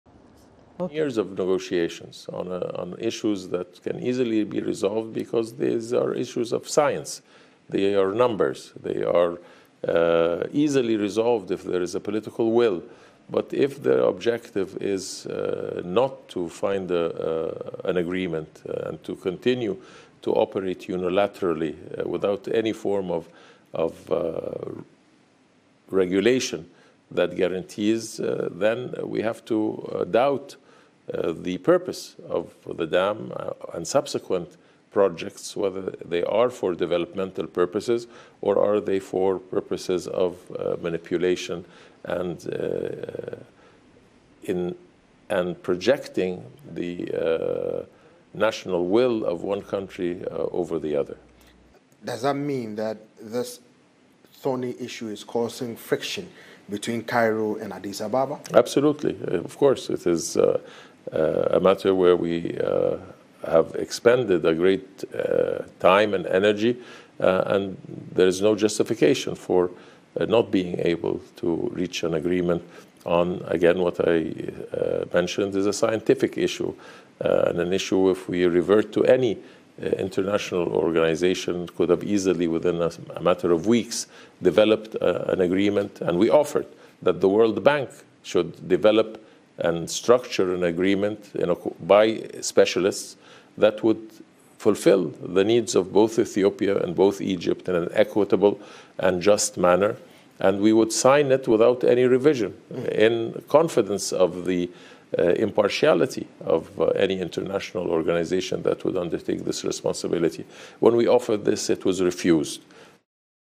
Egypt’s Concerns Mount Over Nile River Dam Construction
Egypt's Foreign Minister, Hassan Shawki, says there is friction between Cairo and Addis Ababa over the continuing construction of the Grand Ethiopian Renaissance Dam. Shawki told VOA's Peter Clottey that the dialogue with Ethiopia to resolve Egypt’s concerns about the use of the Nile River is not meeting expectations.